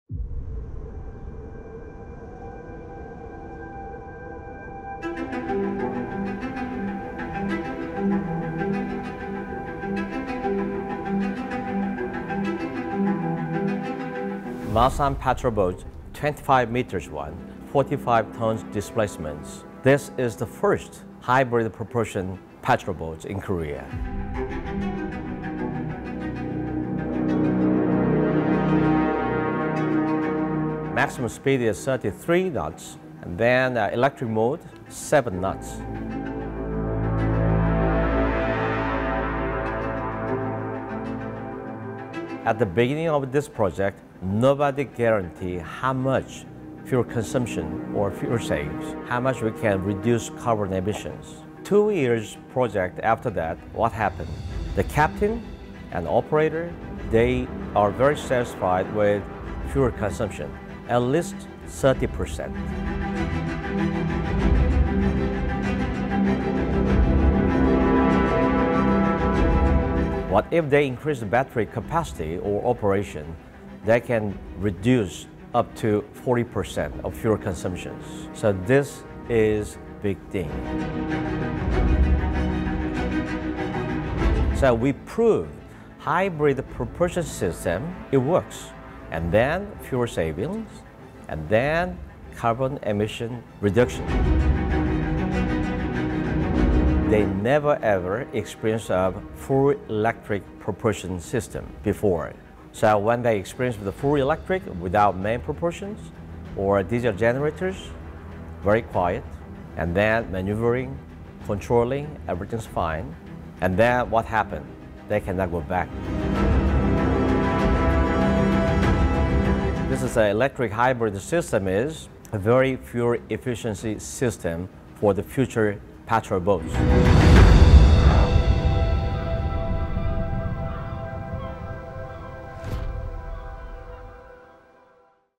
The Patrol Boat Hybrid Electric Propulsion Project
CJ Jeon talks about one of the latest projects JMP Marine has been working on behind the scenes these last few years, Hybrid electric Propulsion South Korean Patrol Boats.